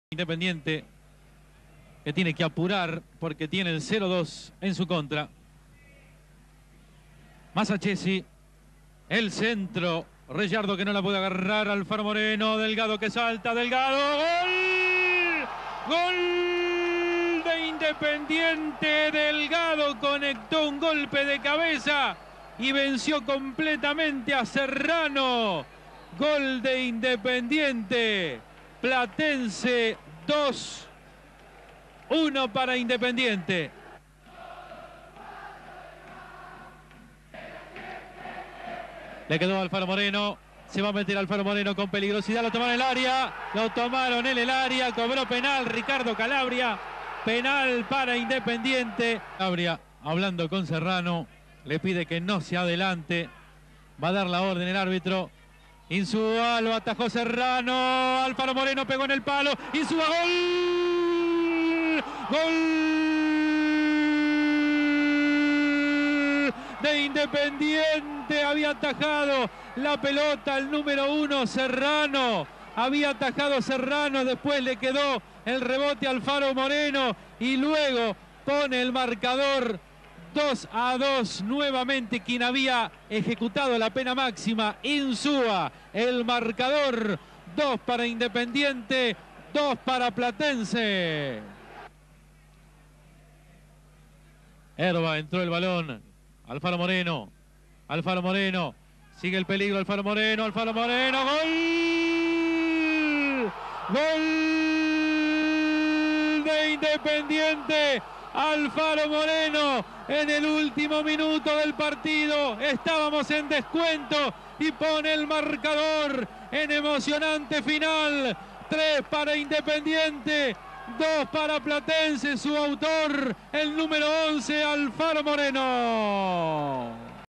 Independiente 3-2 Platense - Torneo 1988/89
26/3/1989 Independiente caía injustamente ante Platense 2-0 pero en la segunda etapa iba a dar vuelta el resultado. Rogelio Delgado, Rubén Insúa y el Beto AlfaroMorneo sobre la hora le dieron el triunfo al Rojo por la 28ª fecha del Torneo